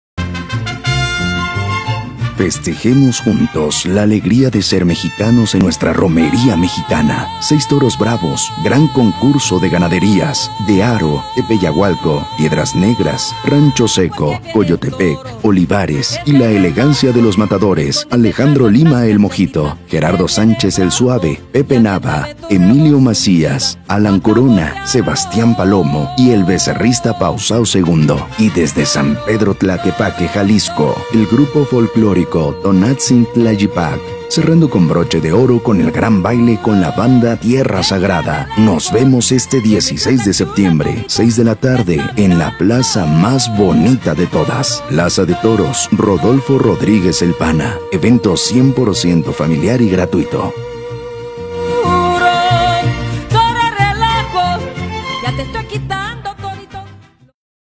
Todo listo en Apizaco para la RomeríaMexicana este 16 de septiembre
Todo listo en Apizaco para la RomeríaMexicana este viernes 16 de septiembre desde las 18:00 horas, en la Monumental Rodolfo Rodríguez “El Pana”.

ENTRADA LIBRE